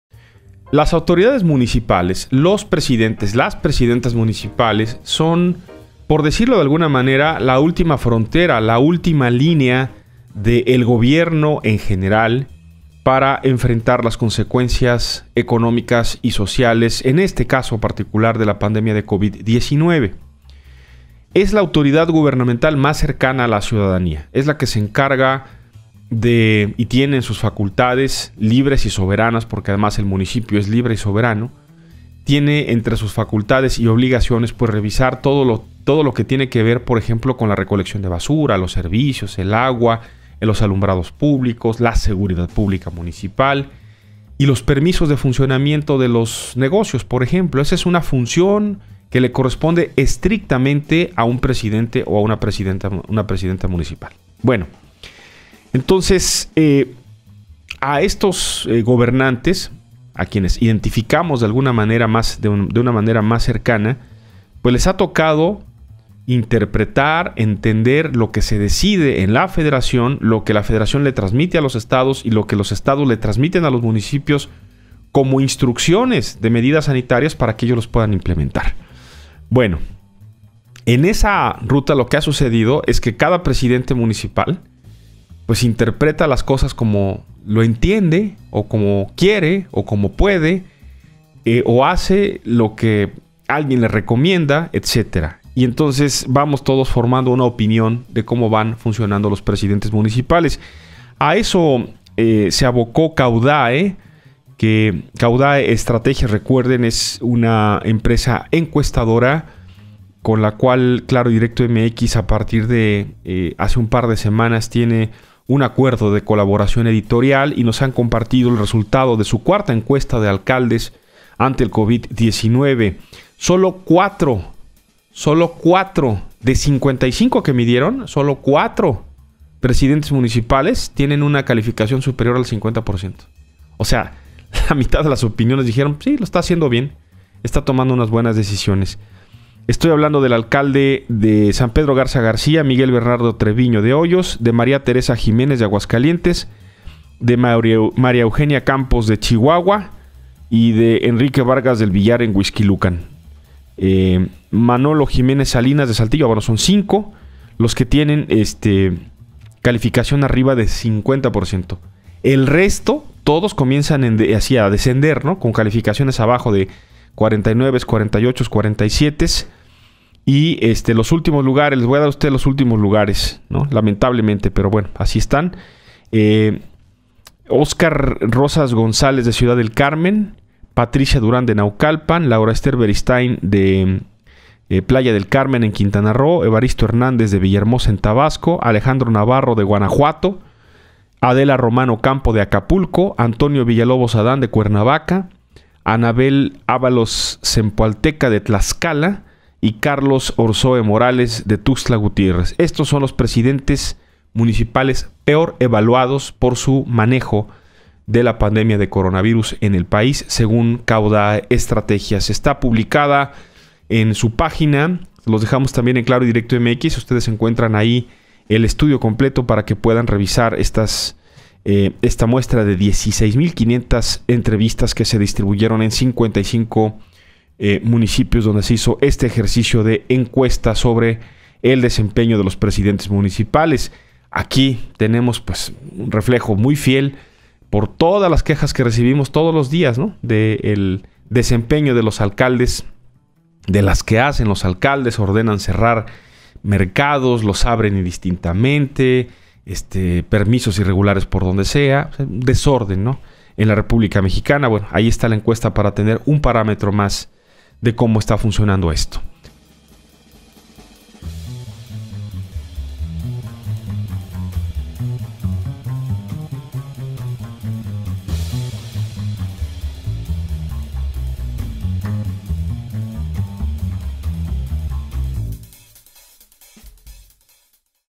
Mal evaluados los presidentes municipales ante la crisis por COVID-19: Encuesta Caudae
Caudae, aplicó la 4a. encuesta de alcaldes ante el COVID-19 y  solo 4 de los 10 mejores evaluados alcanza una aceptación superior al 50%.